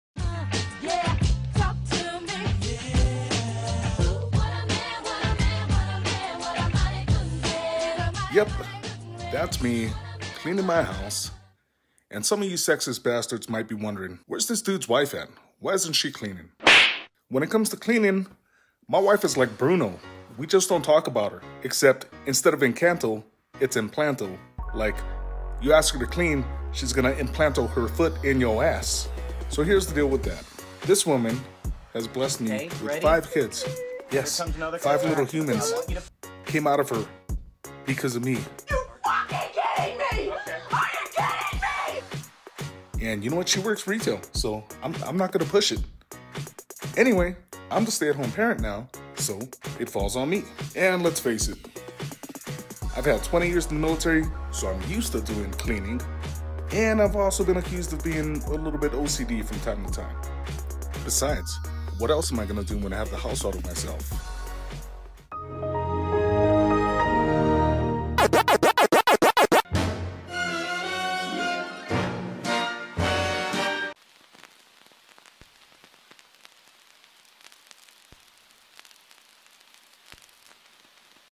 My Stay At Home Dad Journey
Being a stay at home dad of five children definitely had its challenges, but it doesn’t all have to be stressful and serious.  Sometimes you need to just embrace the suck and have a little fun while cleaning and cooking for a family of seven.